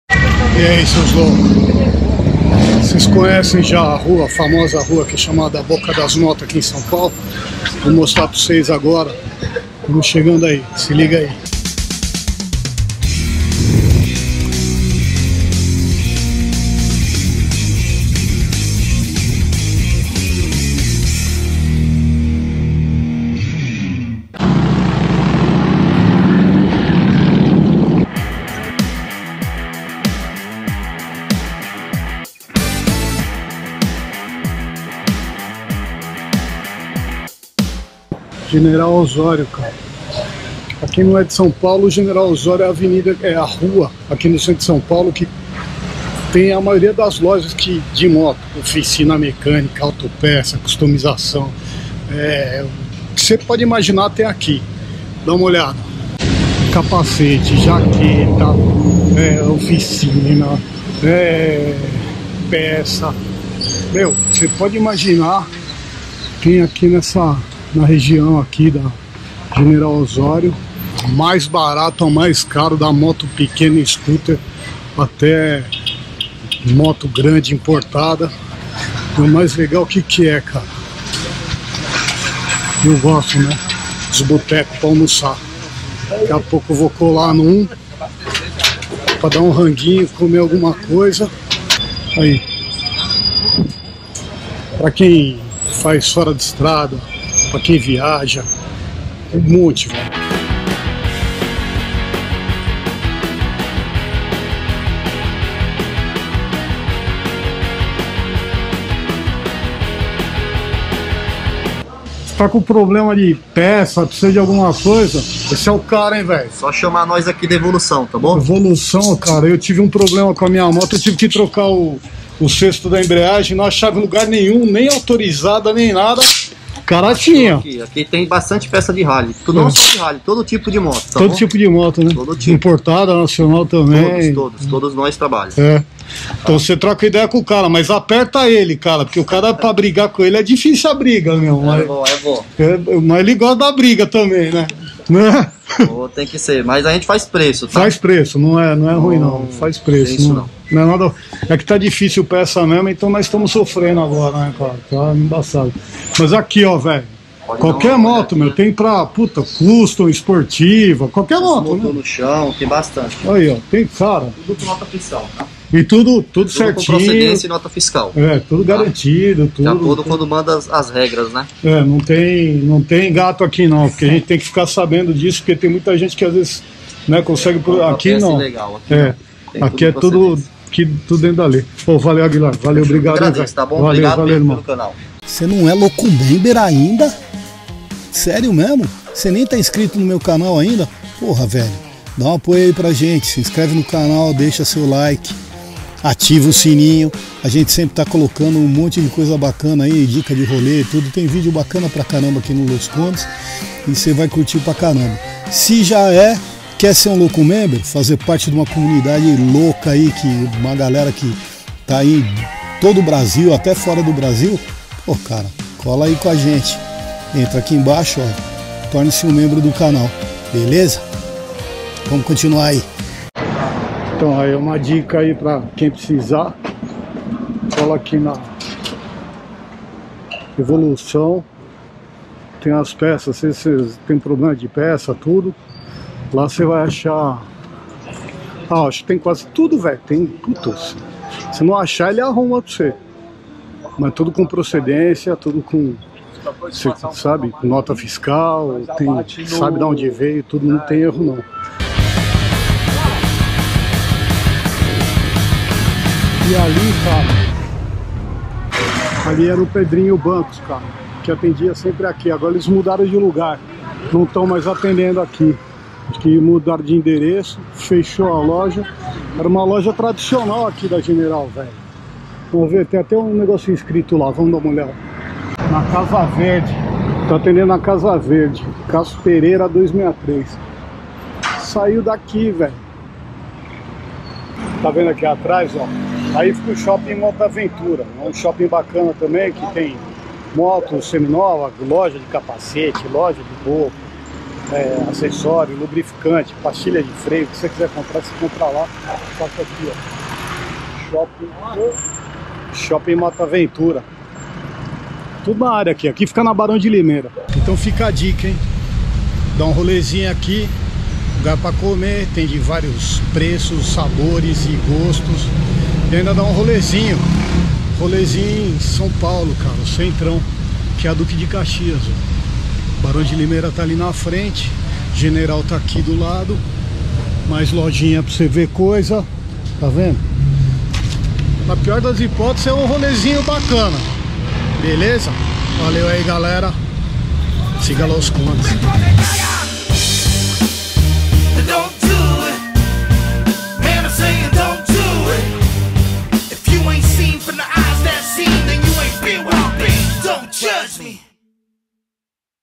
A Boca das Motos
A General Osório é conhecidamente como boca das motos. Lá tudo referente a motos pode ser emcontrado.
De Harley Davidson a antigas importadas até às 125.
Se liga no rolê.

Já é um Membro do Canal? Lo.Co. Member tem muitos benefícios.